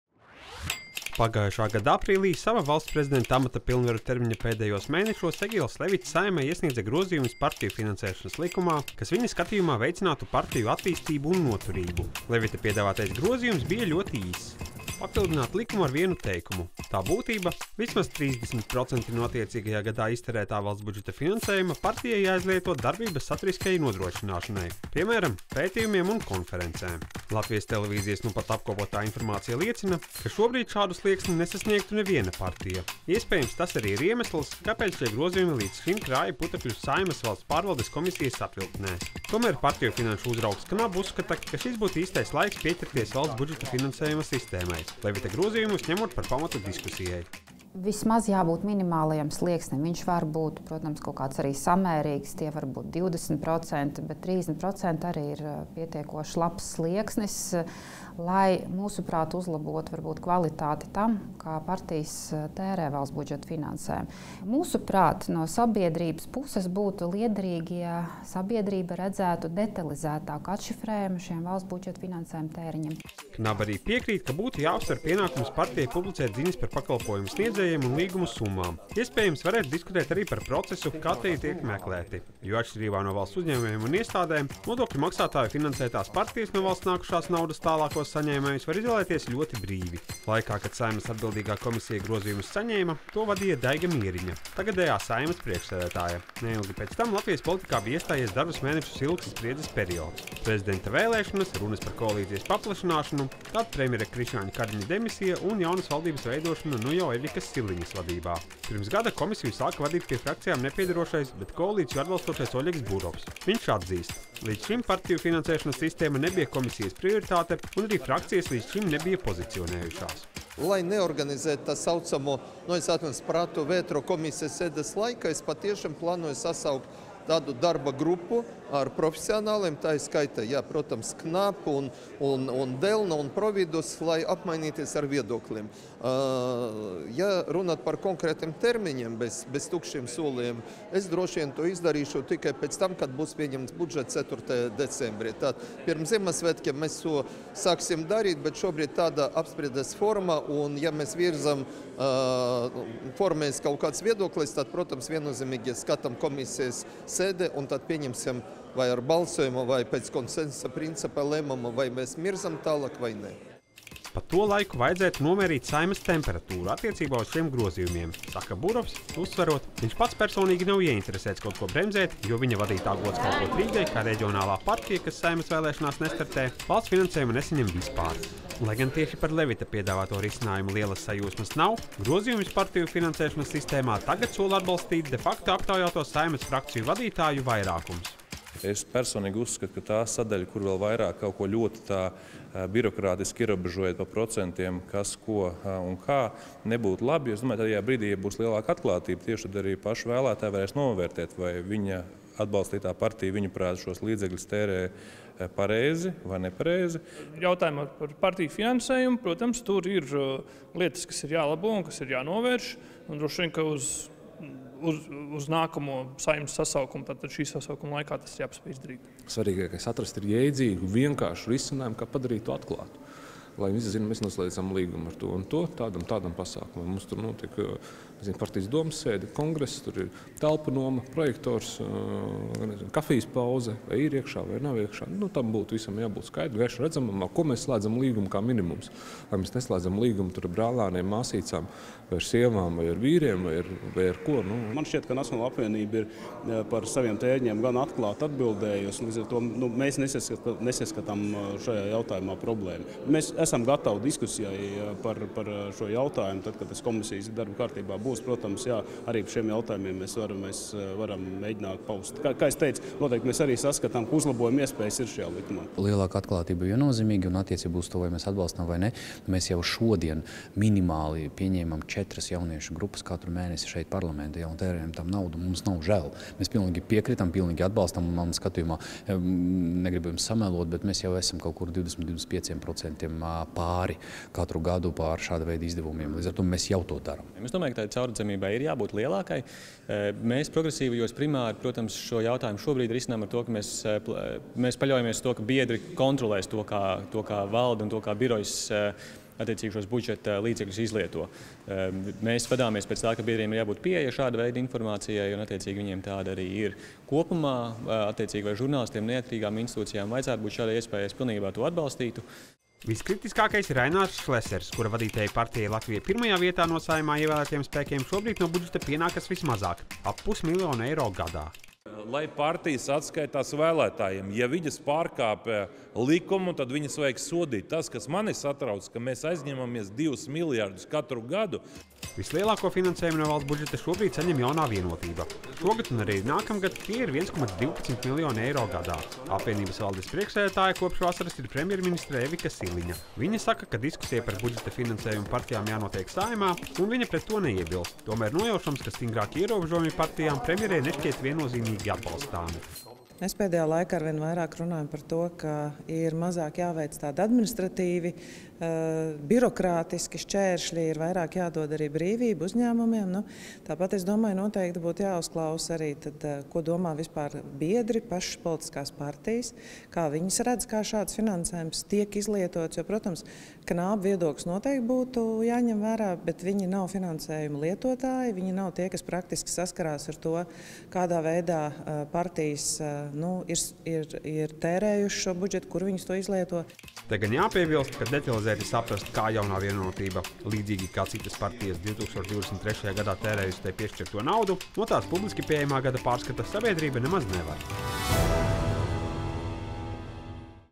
Saeimā sola ķerties pie partiju finansēšanas sistēmas labošanas
Veids, kā politiskās partijas finansē no valsts budžeta, būtu jāuzlabo. Un jāķeras pie šī darba būtu jau tuvākajos mēnešos, lai izmaiņas varētu pieņemt pirms nākamajām Saeimas vēlēšanām. Šāda ir Saeimas frakciju vairākuma nostāja pēc tam, kad “de facto” aktualizēja jautājumu par likuma grozījumiem, ko iesniedza iepriekšējais Valsts prezidents Egils Levits, un kas jau pusotru gadu iestrēguši atbildīgajā komisijā. Partijas uz lielāku atklātību par nodokļu maksātāju naudas izlietošanu mudina arī KNAB.
Valsts prezidenta kancelejā “de facto” pavēstīja, ka Edgars Rinkēvičs, stājoties amatā, ar visām Saeimas frakcijām pārrunājis Levita iesniegto likumprojektu. Tālākā virzība Saeimā ir tās politiskās izšķiršanās jautājums. Prezidenta ieskatā budžeta naudas izlietošana partijām atkarīga gan no likuma burta un gara, gan no tā godprātīgas izpildes. “Normatīvisms veselo saprātu nevar aizstāt,” pauž Rinkēvičs.